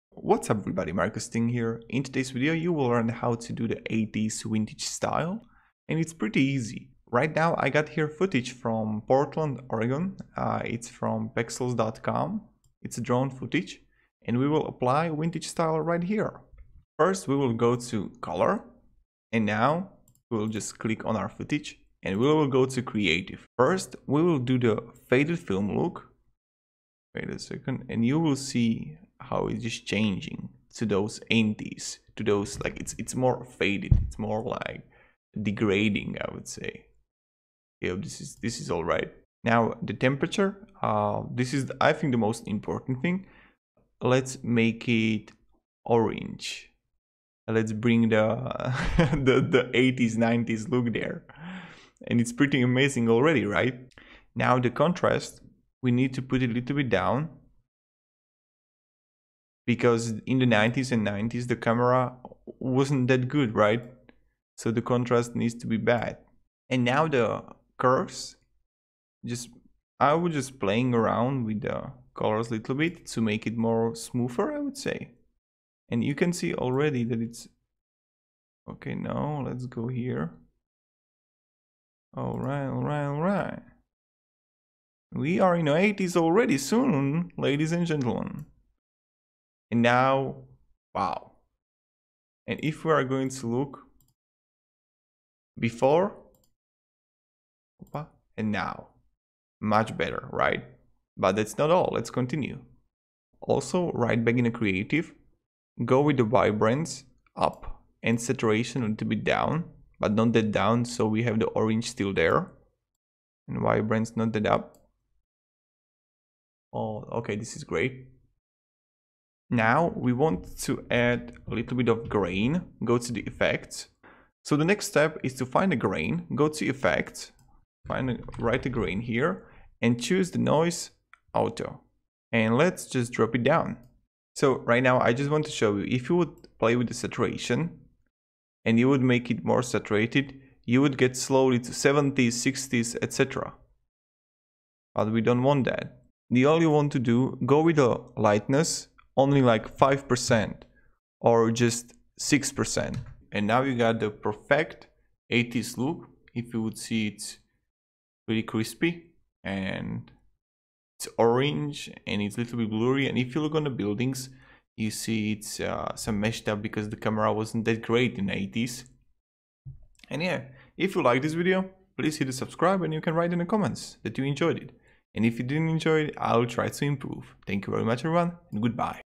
80's Vintage Filter viAdobe Premiere Pro CC
In this tutorial you will learn simple way how you can create vintage 80s vintage color in adobe premiere pro CC. Enjoy :) , transferwise ‏‏‎ ‎

GET AMAZING FREE Tools For Your Youtube Channel To Get More Views:
Tubebuddy (For GROWTH on Youtube):